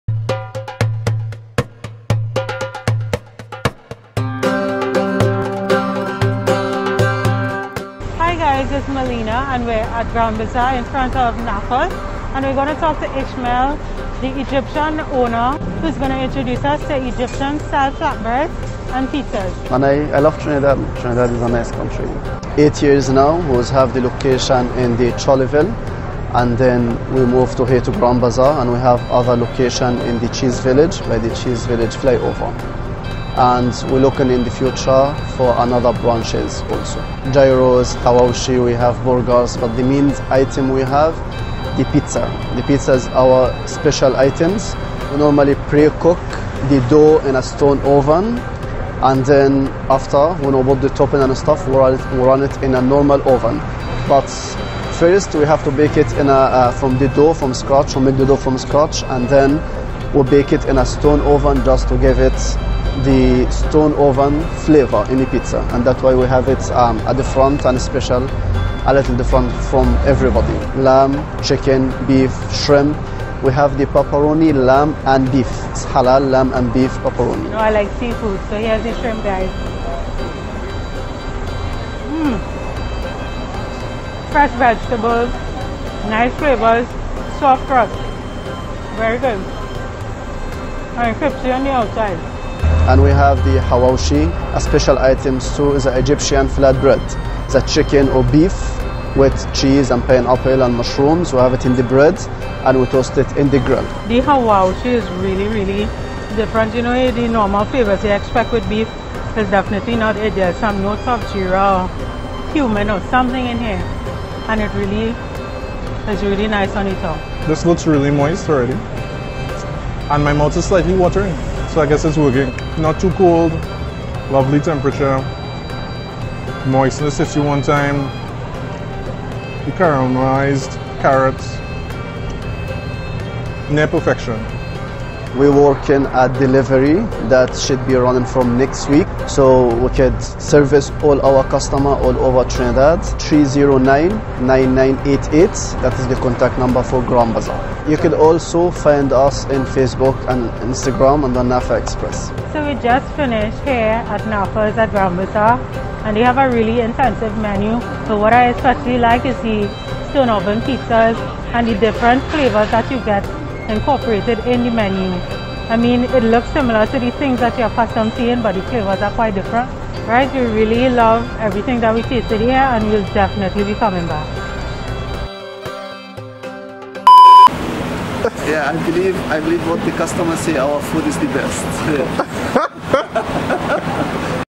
EGYPTIAN PIZZA
As we celebrate 58 years of Independence in Trinidad and Tobago we take the time to reflect on how truly wonderful our country is where every creed and race finds an equal place.
We visited Naafa' Xpress on the Grand Bazzar food strip where we met Ismail who hails from Egypt and runs a business here. He offers a variety of items such as burgers, gyros and even desserts but specializes in brick oven pizza and hawawshi which is Egyptian flatbread that is filled with chicken or beef.
We were eager to know if anything would taste different as the  pizzas and flatbreads looked quite similar to things we have tried before. Surprisingly enough they were very different in taste to how they appear. We opted for a mixed pizza, half chicken half shrimp. The pizza was a nice soft thin crust with crunchy ends and filled with fresh vegetables and topped with shrimp and chicken. The vegetables remained crisp although cooked and the chicken and shrimp was juicy and had it's own unique taste...it was "morish" to tell you the truth. Absolutely no resemblance to that hut or Luigi or those boys pizza. The pizza was clean and didnt have that cheezy oiliness that you normally find in other pizzas.
It was really interesting to taste hawawshi. At first glance it really looks like a quesadilla but the flavors are so very different with a strong presence of cumin, coriander and other spices paired with cheese and grilled to perfection. They also had desserts which are homemade, moist and soooooo good.
Special thanks to Ismail for allowing us to film this episode.
Video by Render Vision Productions